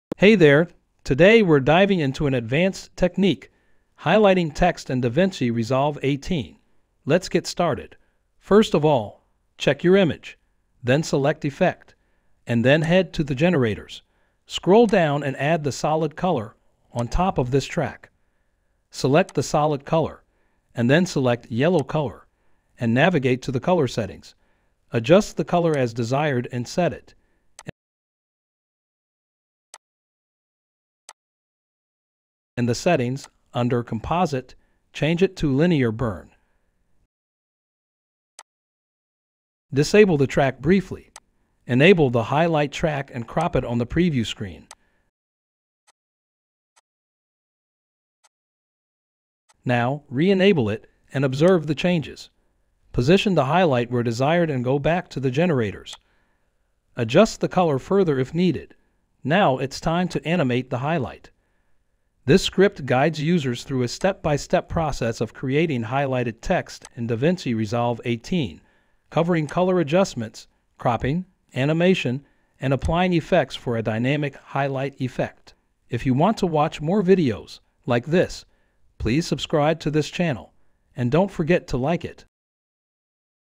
How To Highlight Text In Davinci Resolve 18 | IN 1 MIN Text Highlight Davinci Resolve
In this quick tutorial, we'll show you how to highlight text in DaVinci Resolve 18 in just 1 minute. Adding text highlights can make your video content more engaging and informative, and we'll guide you through it